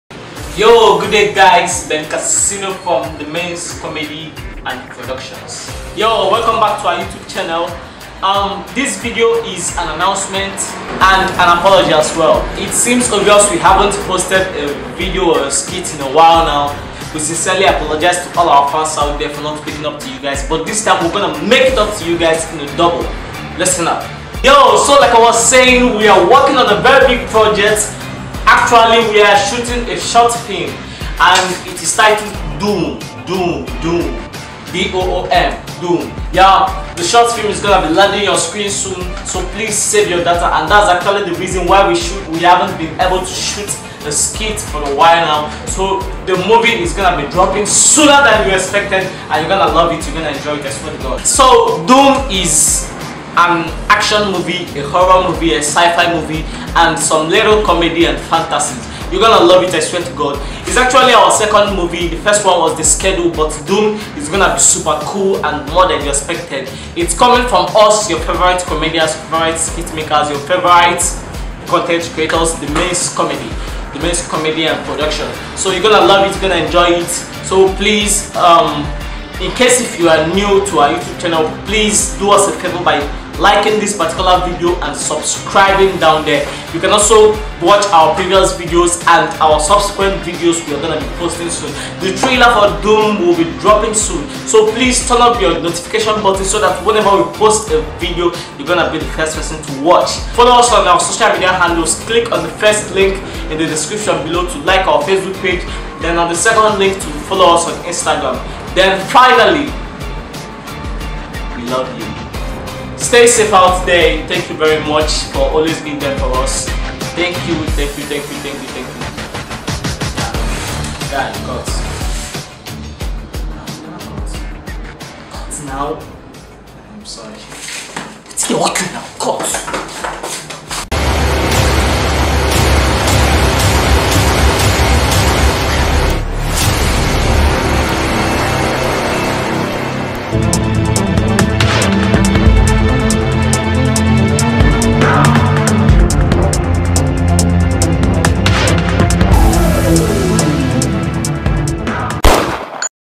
ANNOUNCEMENT! (De Mainz Rangerz)
Good day Good people🌏🇳🇬
We sincerely apologise for not posting a skit in a while now...
And that's because we are working on a short film..
It's gonna be hitting ur screens soon.